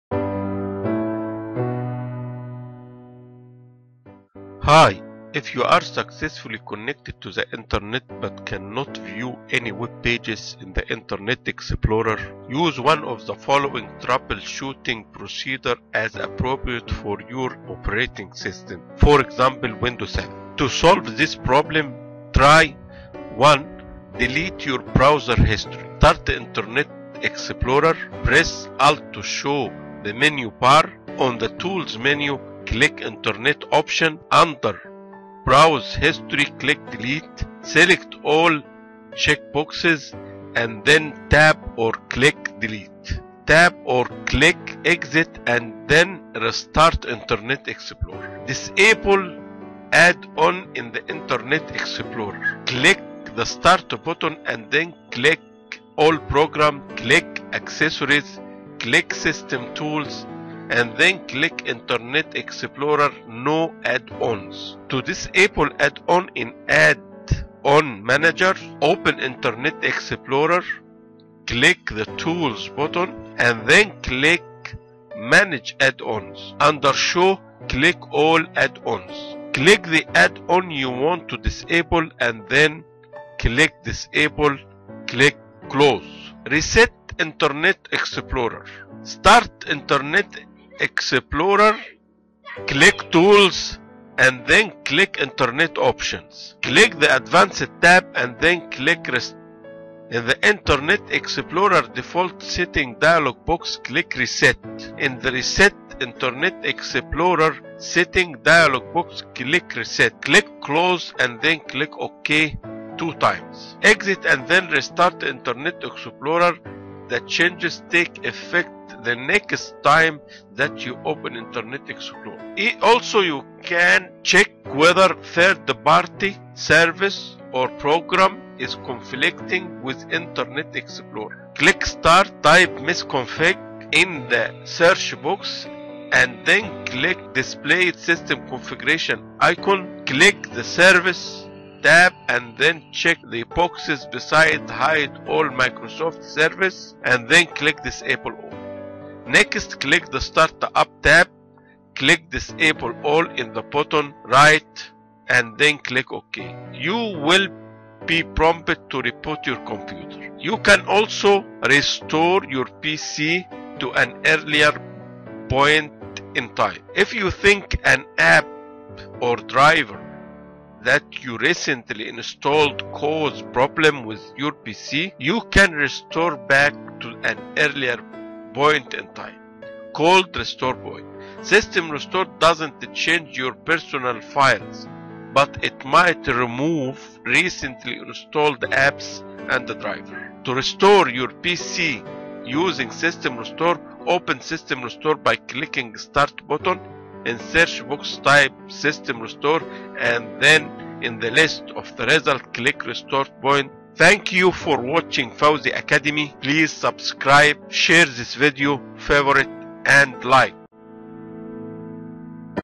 Internet Explorer cannot display the Webpage FIX
"Internet Explorer cannot display the webpage" error
Delete your browser history
Reset Internet Explorer
Restore your PC to an earlier point in time